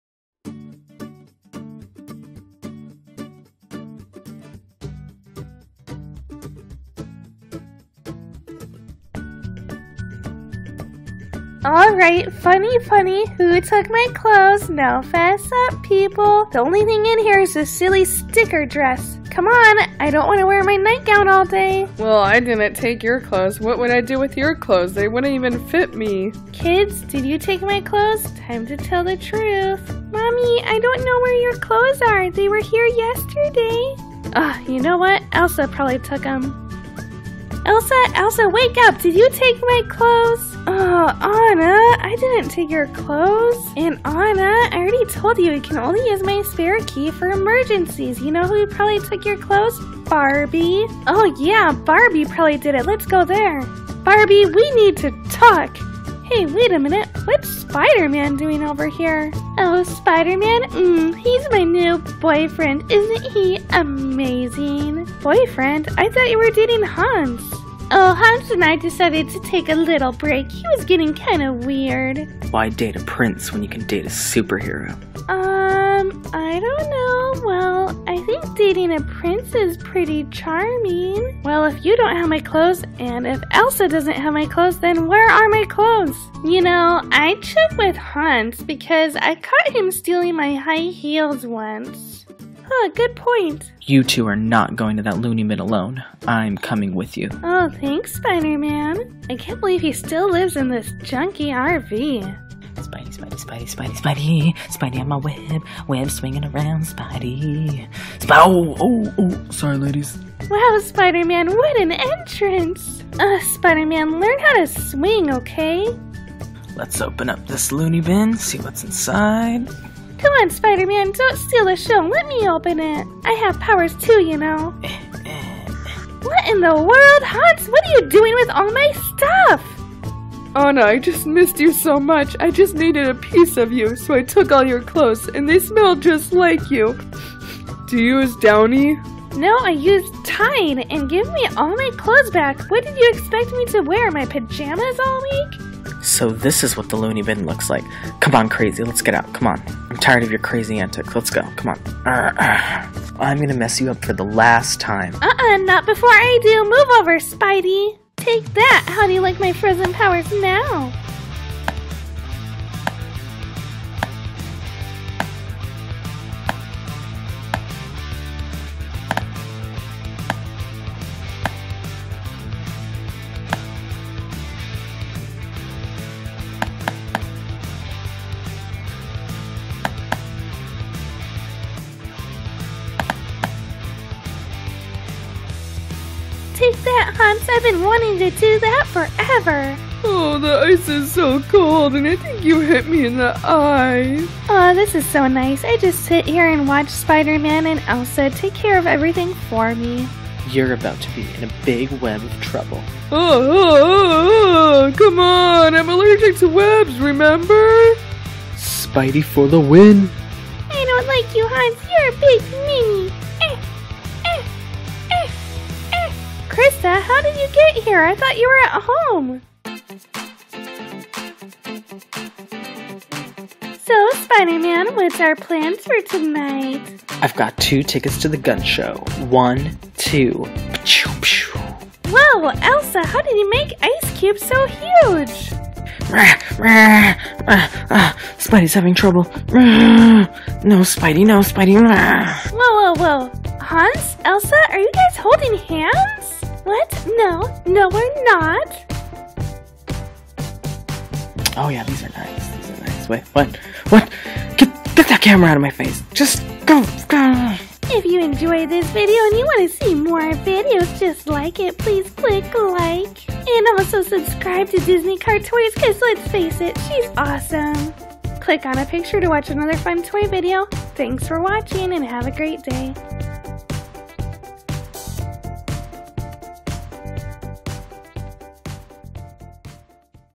Anna Kidnapped! ❤ Part 4! Frozen Hans Takes Anna s Clothes Elsa & Spider Man Fight Hans
children's entertainment | Anna Kidnapped! ❤ Part 4! Frozen Hans Takes Anna s Clothes Elsa & Spider Man Fight Hans